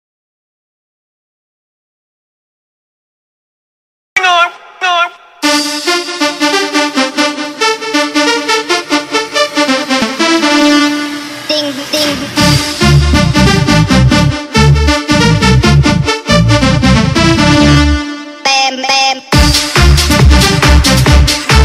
Bomba de Fabão
Com um milagre, a 35m Fabão Meteu o canudo e fez um golaço.